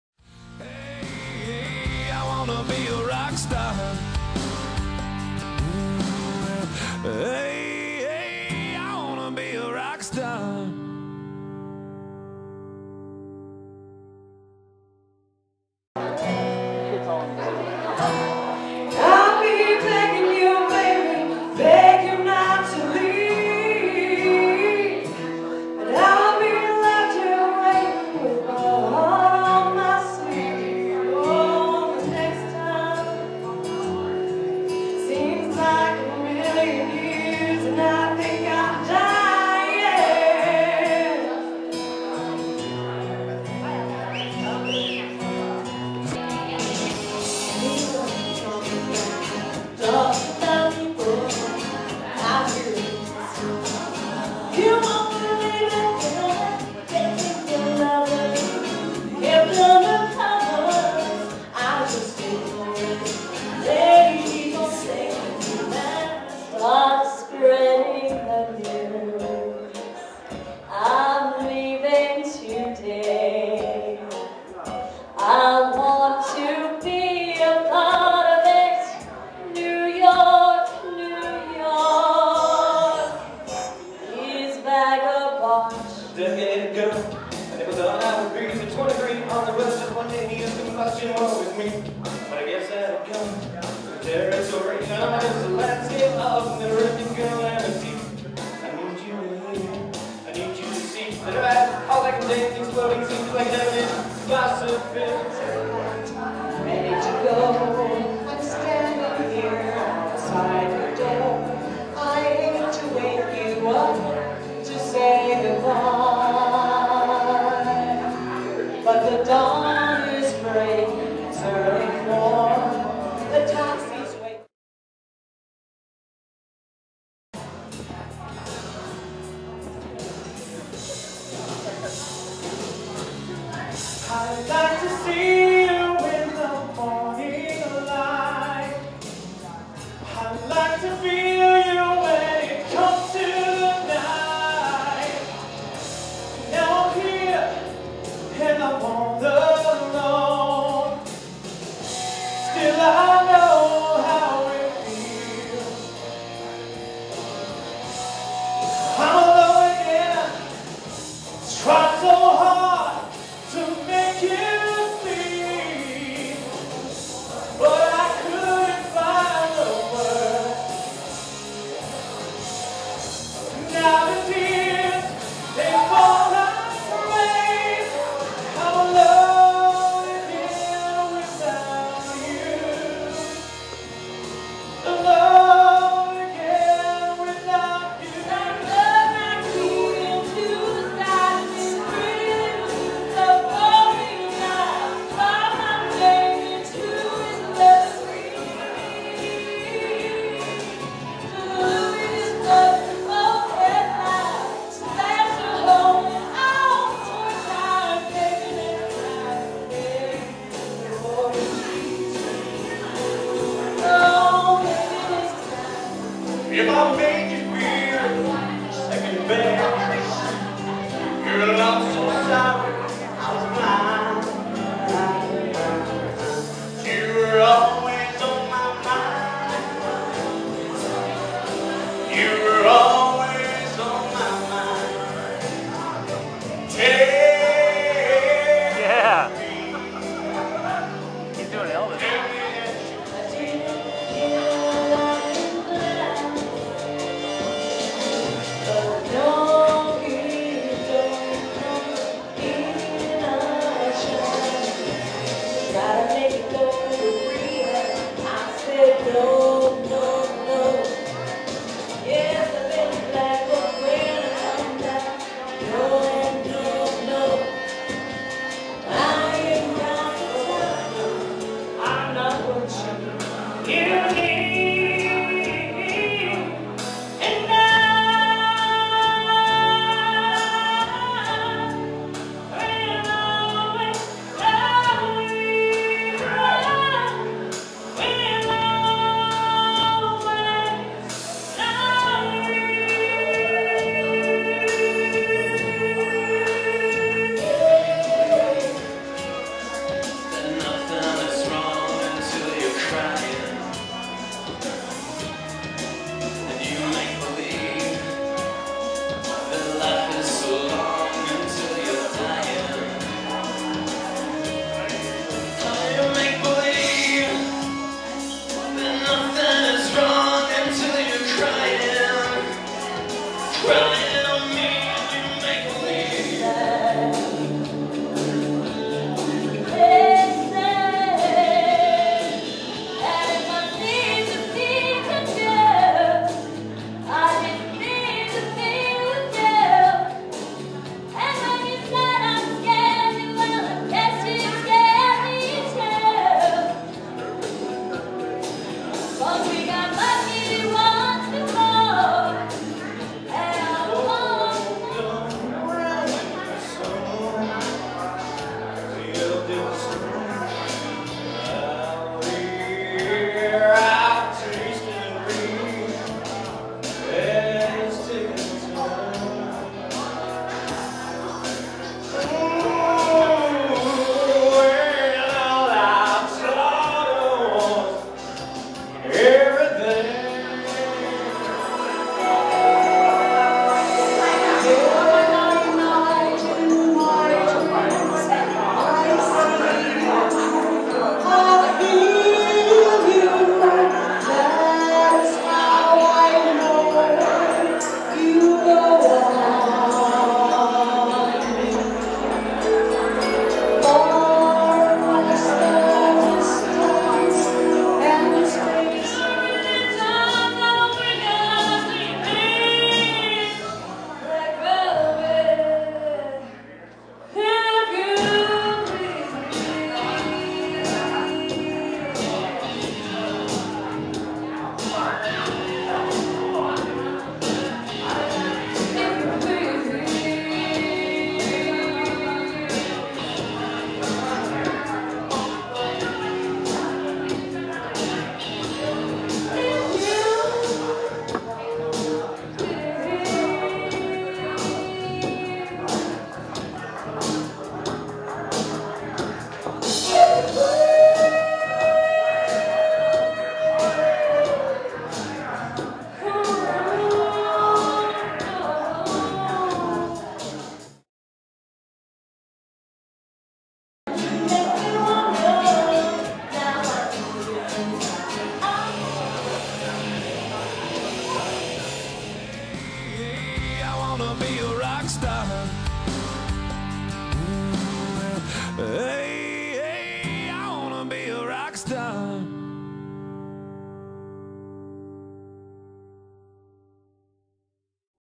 American Icon Week 4
American Icon Karaoke is a competition running every Wednesday night at Fox and The hound. DJ Grumpy Old Man filmed by Beer & Camera Enjoy.